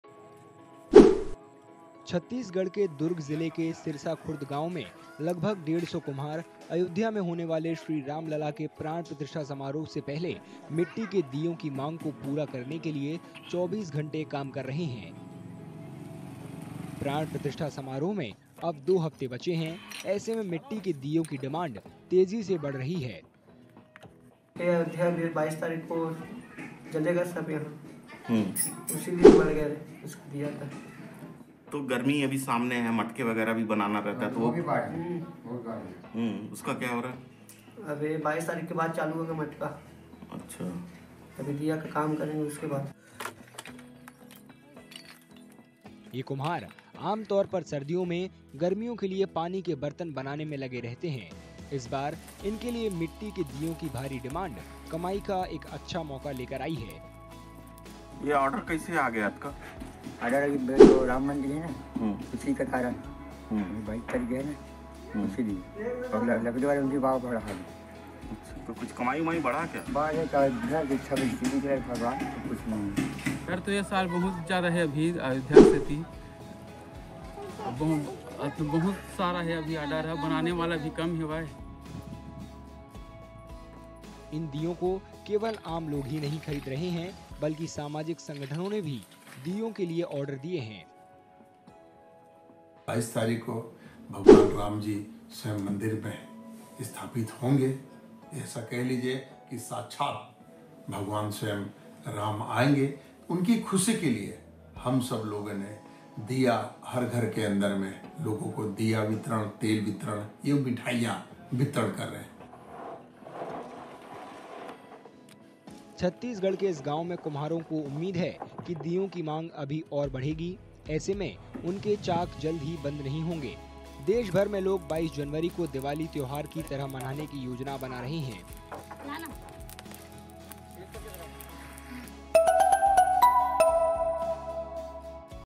छत्तीसगढ़ के दुर्ग में कुम्हार 'दीयों' की भारी मांग को पूरा करने के लिए 24 घंटे कर रहे हैं काम
छत्तीसगढ़ के दुर्ग जिले के सिरसा खुर्द गांव में लगभग 150 कुम्हार, अयोध्या में होने वाले श्रीराम लला के प्राण प्रतिष्ठा समारोह से पहले मिट्टी के दीयों की मांग को पूरा करने के लिए 24 घंटे काम कर रहे हैं। प्राण प्रतिष्ठा समारोह में अब दो हफ्ते बचे हैं। ऐसे में मिट्टी के दीयों कि डिमांड तेजी से बढ़ रही है। ये कुम्हार आमतौर पर सर्दियों में, गर्मियों के लिए पानी के बर्तन बनाने में लगे रहते हैं। इस बार इनके लिए मिट्टी के दीयों की भारी डिमांड, कमाई का एक अच्छा मौका लेकर आई है।